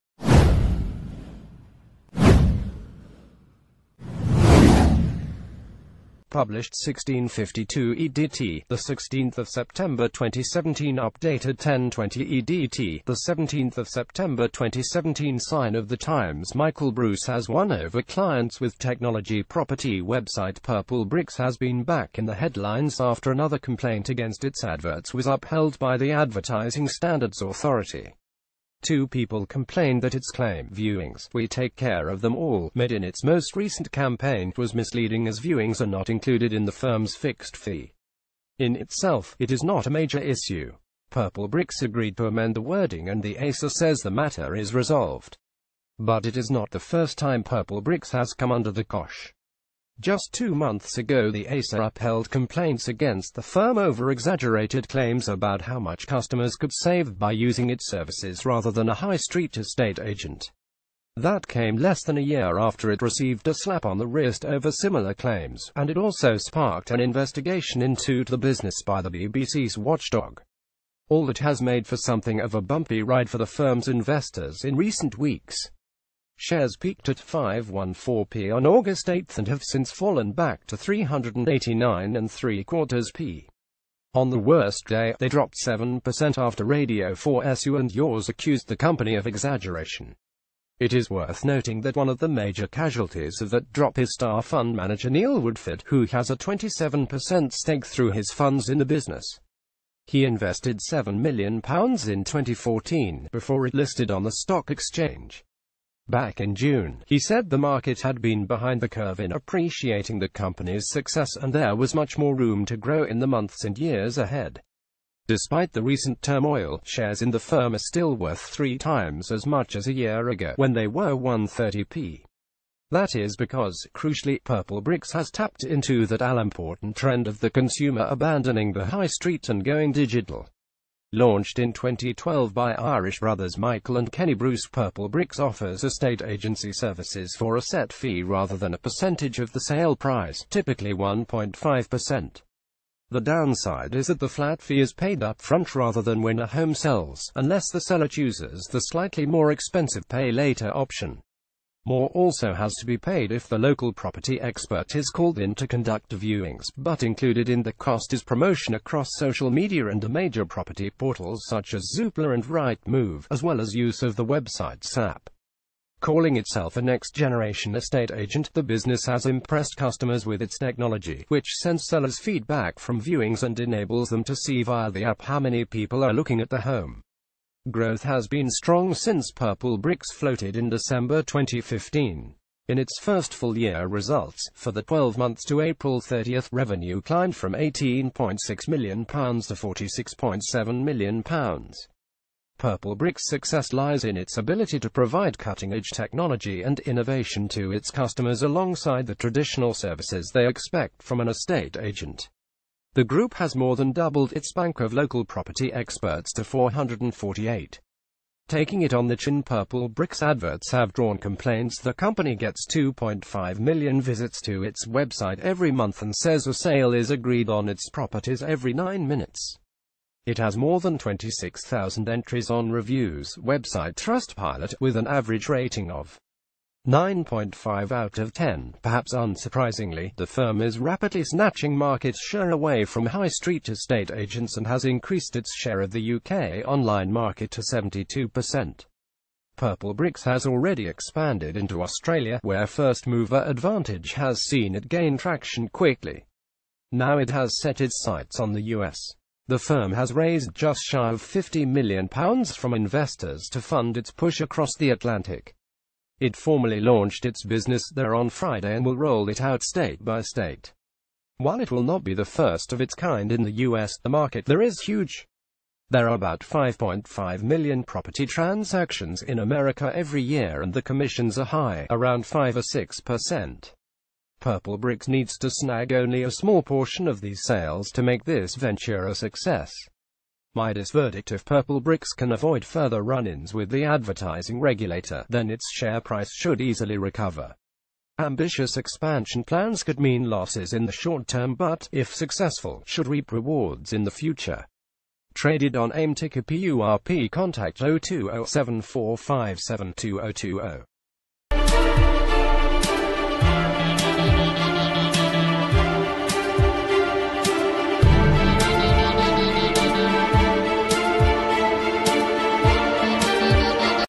Breaking News | Midas share tips: purplebricks is set for us push
Midas share tips: purplebricks is set for us push

Published 1652 EDT, 16 September 2017 Updated 1020 EDT, 17 September 2017 Sign of the times Michael ...